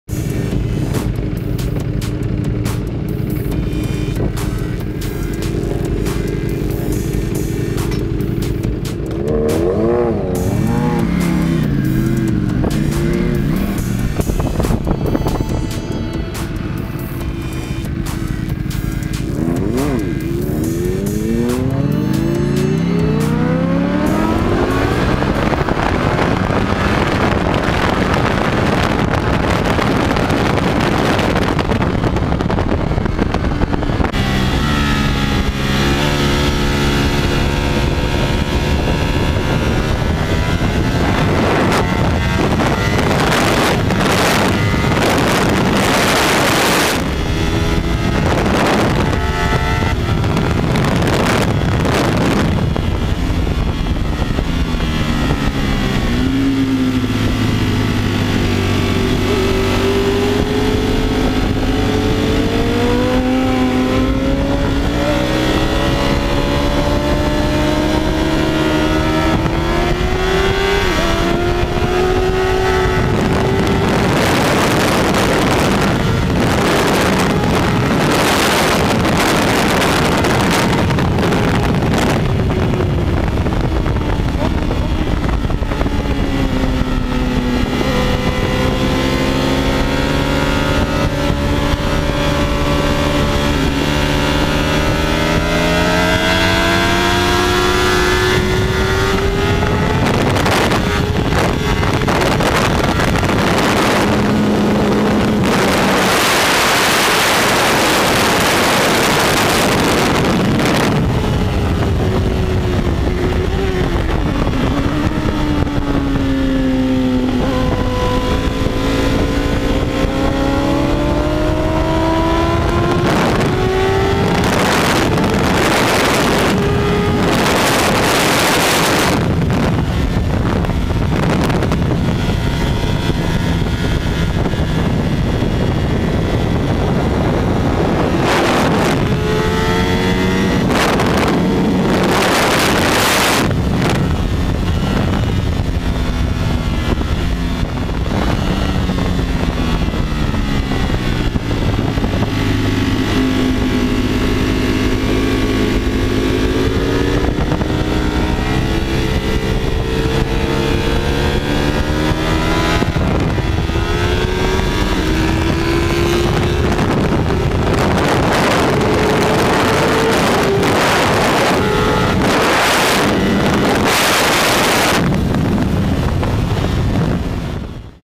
Slovakiaring Panigale v4S and friends /
len tak z ľahka...  Ďaľšia jazda už z preteku pohľady z prednej aj zadnej kamery - klikni na link nižšie 1. miesto Slovakia ring z BIKES PLANET RACING CUP :